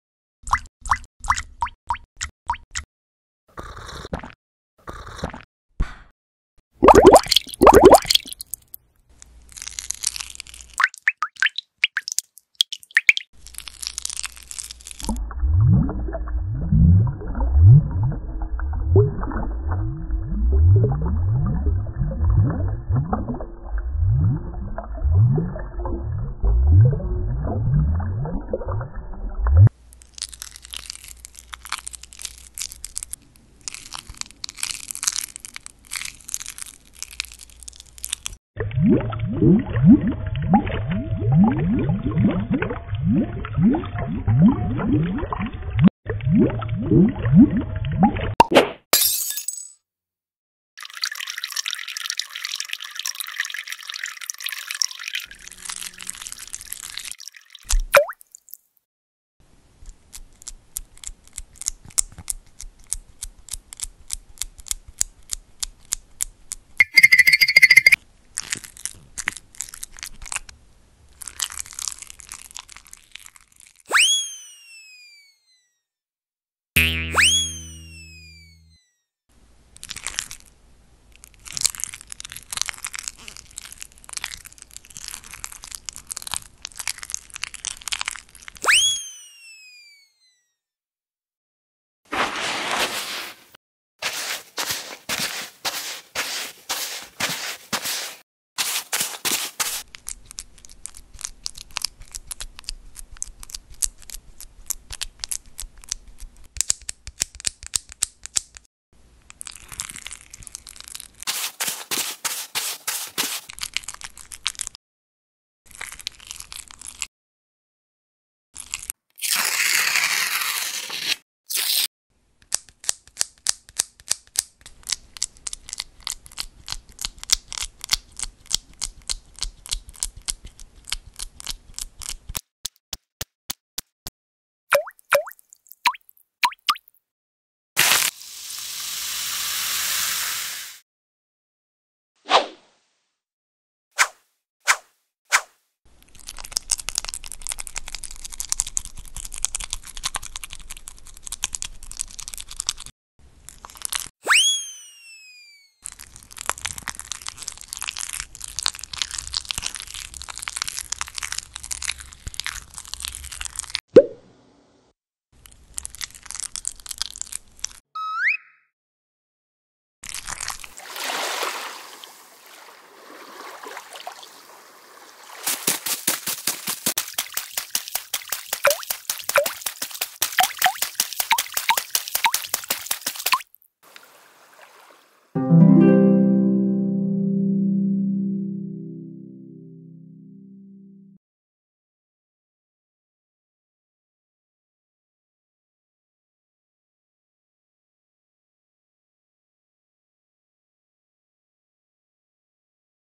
Satisfying Slime ASMR Cute Animation | Slimy Boba Stuck on Tape
Boba Marumaru embarks on an adventure, and his tiny body creates soothing, satisfying slime sounds along the way. Relax and enjoy the fun, squishy journey!